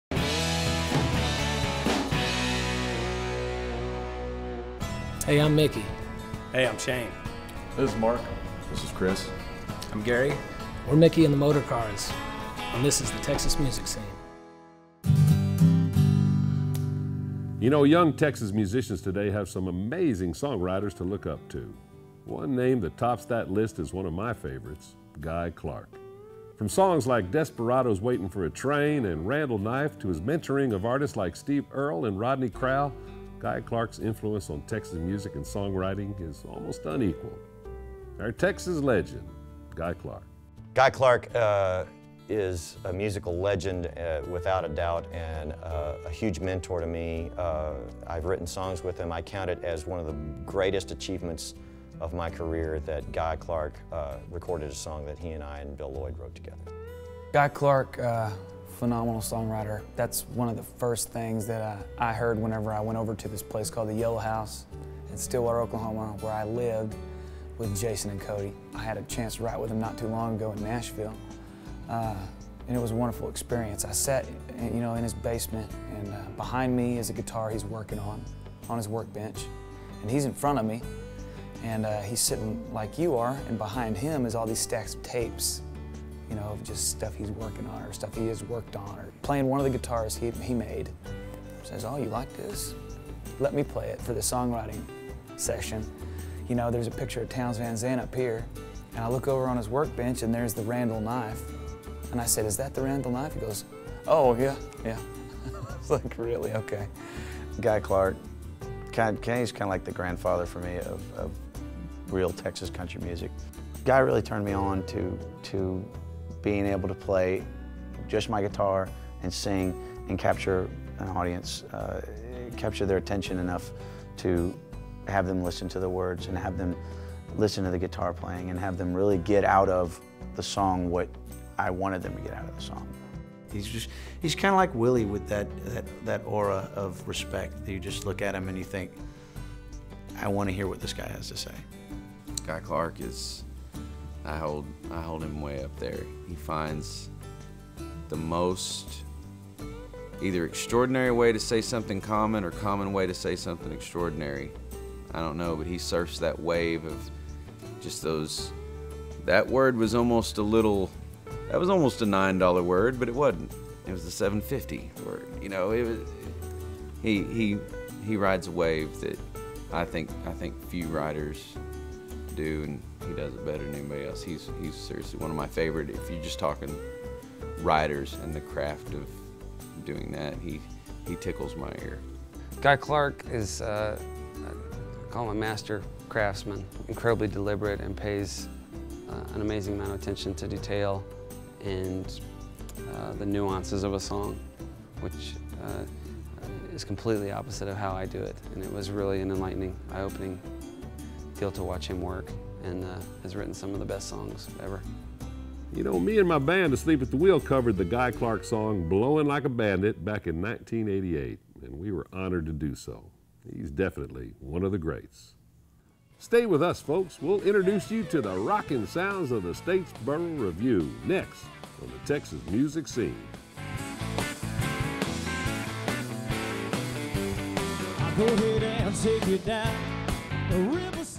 Texas Legend - Guy Clark
A variety of Texas artists talk about the influence of Guy Clark on their songwriting and their lives.